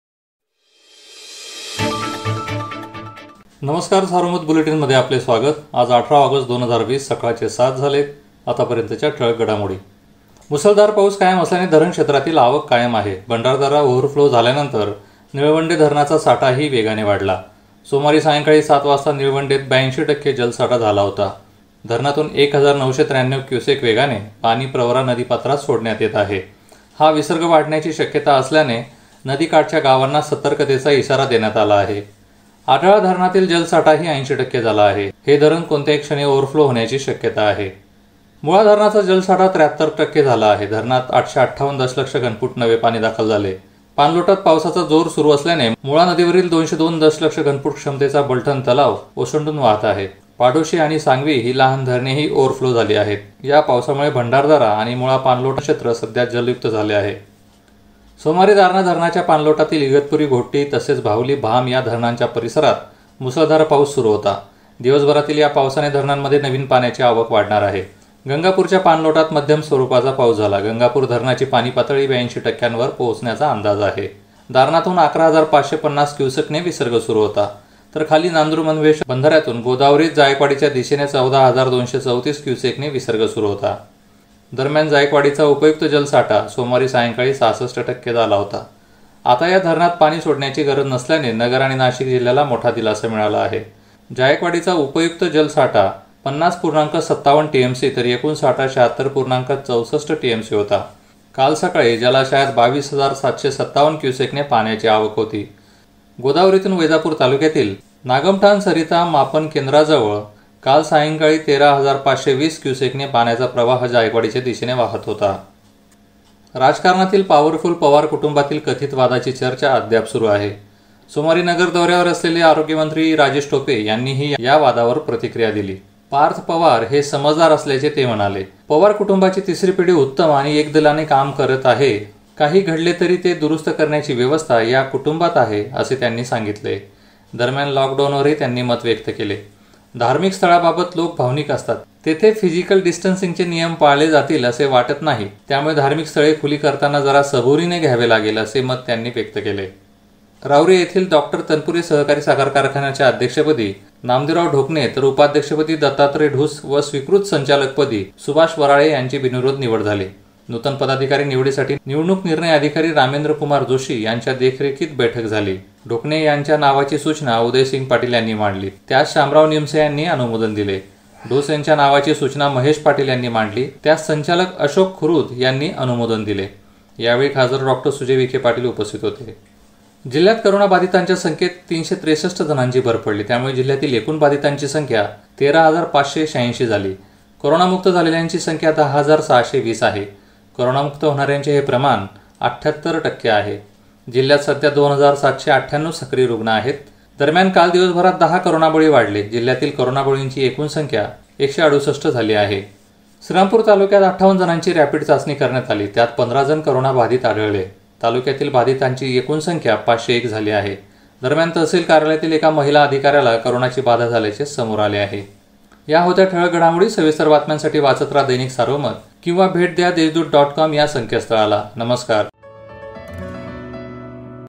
निळवंडेतून प्रवरेत विसर्ग वाढणार | bhandardara | nilwande | mula | darana | jayakwadi
- मुसळधार पाऊस कायम असल्याने धरण क्षेत्रातील आवक कायम आहे.  भंडारदरा ओव्हरफ्लो झाल्यानंतर निळवंडे धरणाचा साठाही वेगाने वाढला.  सोमवारी सायंकाळी 7 वाजता निळवंडेत 82 टक्के जलसाठा झाला होता. धरणातून 1 हजार 993 क्युसेक वेगाने पाणी प्रवरा नदीपात्रात सोडण्यात येत आहे.
- मुळा धरणाचा जलसाठा 73 टक्के झाला आहे. धरणात 858 दशलक्ष घनफूट नवे पाणी दाखल झाले.

-  दारणा धरणाच्या पाणलोटातील इगतपुरी, घोटी तसेच भावली,  भाम या धरणांच्या परिसरात मुसळधार पाऊस सुरु होता. दिवसभरातील या पावसाने धरणांमध्ये नवीन पाण्याची आवक वाढणार आहे.
- जायकवाडीचा उपयुक्तसाठा सोमवारी सायंकाळी 66 टक्के झाला होता. आता या धरणात पाणी सोडण्याची गरज नसल्याने ahmednagar nashik जिल्हयाला मोठा दिलासा मिळाला आहे.

राजकारणातील पावरफुल पवार कुटुंबातील sharad pawar कथित वादाची चर्चा अद्याप सुरू आहे. सोमवारी नगर दौर्‍यावर असलेले आरोग्यमंत्री rajesh tope यांनीही या वादावर प्रतिक्रीया दिली. पार्थ पवार parth pawar समजदार असल्याचे ते म्हणाले.

rahuri येथील डॉ. तनपुरे साखर कारखान्याच्या अध्यक्षपदी नामदेवराव ढोकणे तर उपाध्यक्षपदी दत्तात्रय ढूस व स्वीकृत संचालकपदी सुभाष वराळे यांची बिनविरोध निवड झाली.

जिल्ह्यात coronavirus बाधितांच्या संख्येत 363 जणांची भर पडली. यामुळे जिल्ह्यातील एकूण बाधितांची संख्या 13 हजार 586 झाली. दरम्यान, काल दिवसभरात 10 करोनाबळी वाढले.

श्रीरामपूर तालुक्यात 58 जणांची रॅपीड चाचणी करण्यात आली. त्यात 15 जण करोना बाधित आढळले.